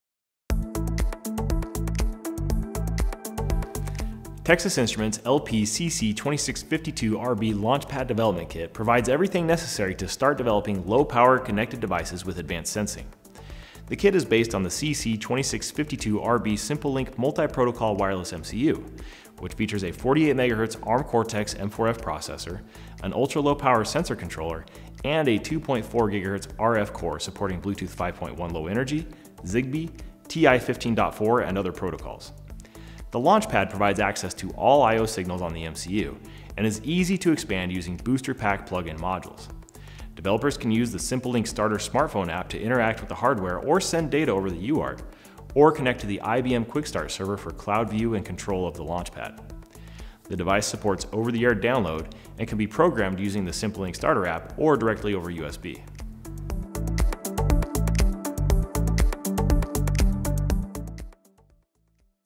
TEXAS INSTRUMENTS LP-CC2652RB LaunchPad Development Kit | New Product Brief
Texas Instruments LP-CC2652RB LaunchPad Development Kit provides everything necessary to develop low-power connected devices with advanced sensing. The kit is based on the CC2652RB SimpleLink™ multiprotocol wireless MCU, which features a 48MHz Arm Cortex-M4F processor, an ultra-low-power sensor controller, and a 2.4GHz RF core supporting Bluetooth 5.1 Low Energy, ZigBee, TI 15.4, and other protocols.